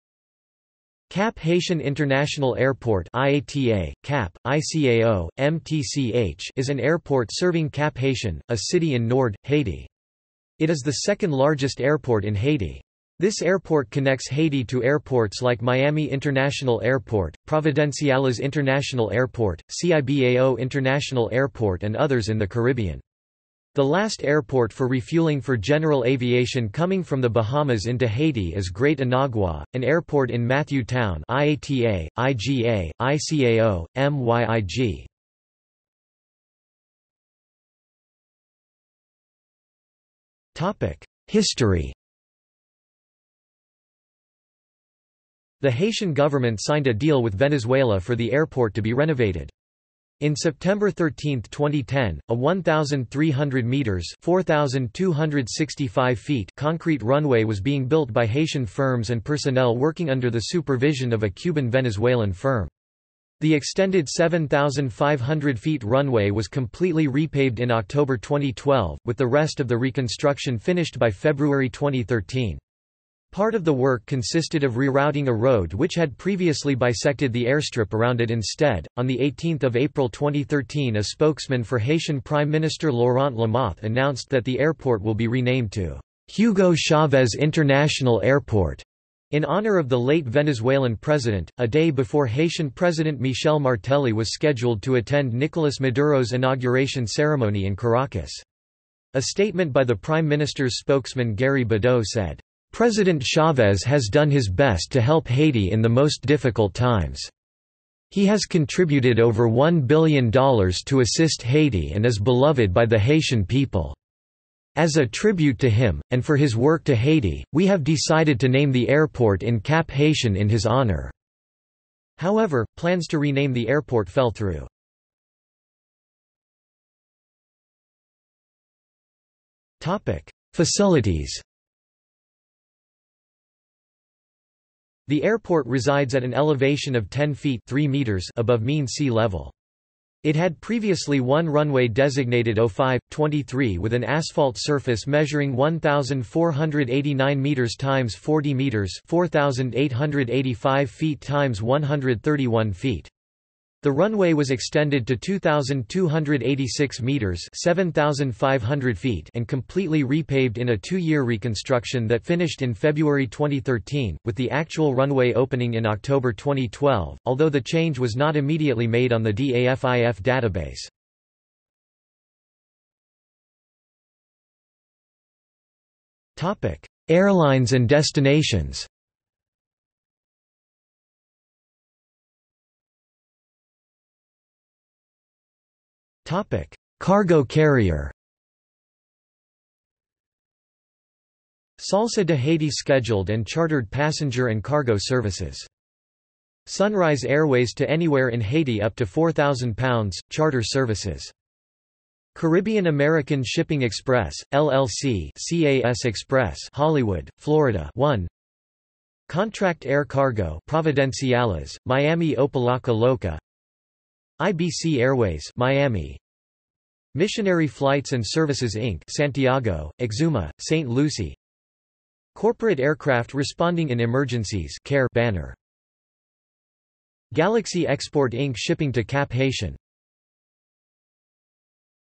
Cap-Haïtien International Airport | Wikipedia audio article
00:00:36 1 History
00:02:06 2 Facilities
00:02:52 3 Airlines and destinations
00:03:02 4 Cargo Carrier

- Socrates

SUMMARY
Cap-Haïtien International Airport (IATA: CAP, ICAO: MTCH) is an airport serving Cap-Haïtien, a city in Nord, Haiti. It is the second largest airport in Haiti. This airport connects Haiti to airports like Miami International Airport, Providenciales International Airport, Cibao International Airport and others in the Caribbean. The last airport for refueling for general aviation coming from the Bahamas into Haiti is Great Inagua, an airport in Matthew Town (IATA: IGA, ICAO: MYIG).